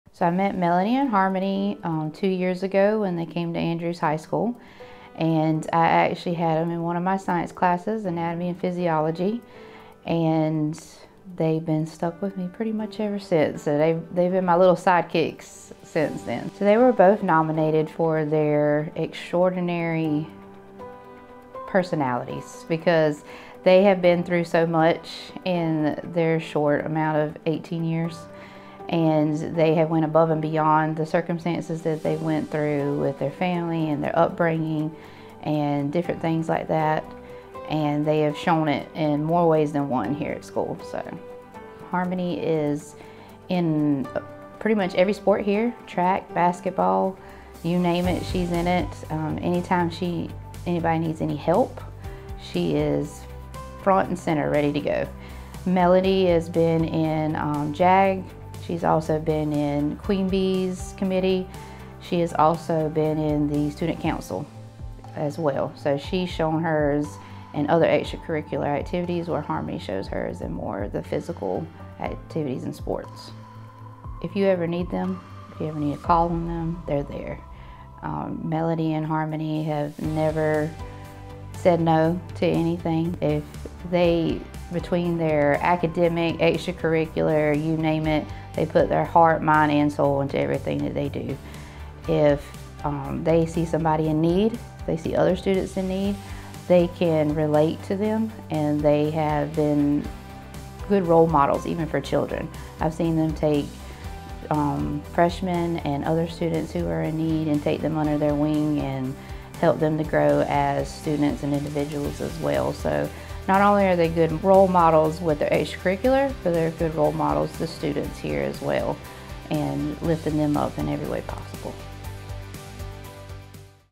HTC REEL Kids: Melody & Harmony - Andrews High School
Melody and Harmony, twin sisters who attend Andrews High, have overcome much in their young lives. From the moment they were born, Melody and Harmony have been fighting battles, great and small. But each time they have prevailed. Every triumph has made them stronger and more confident.
Born in 2005, the pair faced their first battle by spending two months in the NICU due to complications from being born early. Since then, they have struggled with consistent battles throughout their eighteen years. Moving from place to place, being placed in foster care twice and being separated from their mother for 10 years, By the time they were introduced into public school, COVID had turned the world upside down, making the transition into high school exceedingly difficult. Most students their age have not faced even half of what Melody and Harmony have endured in their short lifetimes.